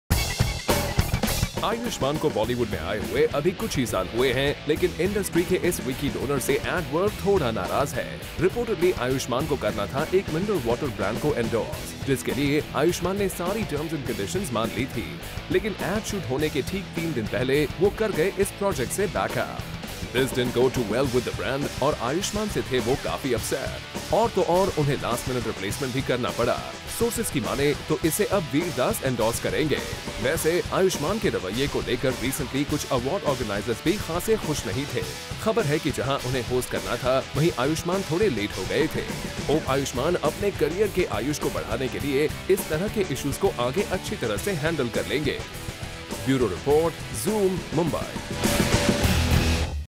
Ayushmann Khurrana's stary tantrums
Rumour has it that the new kid on the block rejected an endorsement forcing the company to find someone new at the last minute.